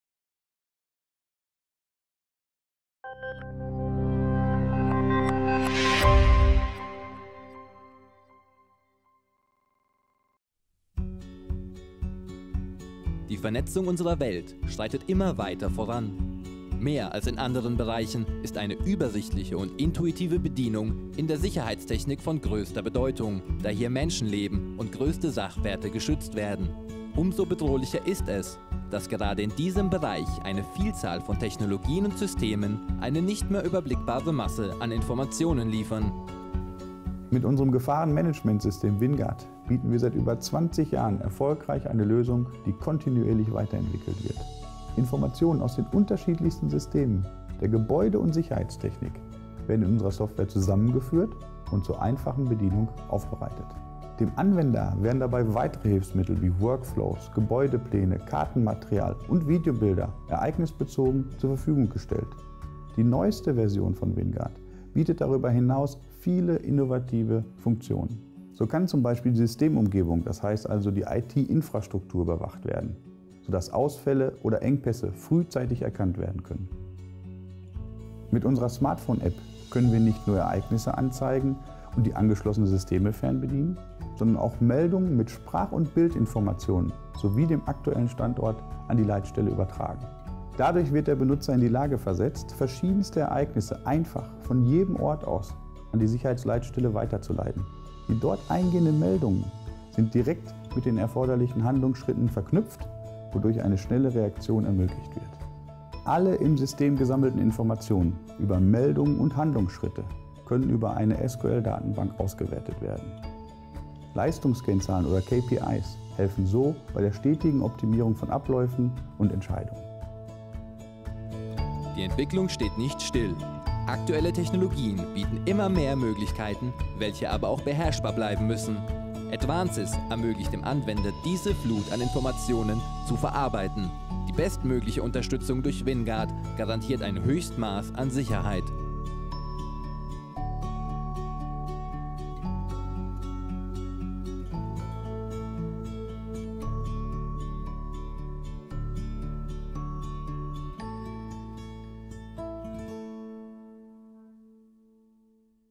Advancis PSIM+ Software Lösungen [Deutsch]
Untertitel : Deutsch, Englisch, Niederländisch, Französisch
Beschreibung : Die Vernetzung unserer Welt schreitet immer weiter voran. Gerade im Bereich der Sicherheitstechnik liefert eine Vielzahl von Technologien eine unüberblickbare Masse an Informationen. Mit unserem Gefahrenmanagementsystem WinGuard bieten wir seit über 20 Jahren erfolgreich eine Lösung, die es dem Anwender ermöglicht, diese Flut an Informationen zu verarbeiten. Dabei garantiert die umfassende Unterstützung durch WinGuard ein Höchstmaß an Sicherheit.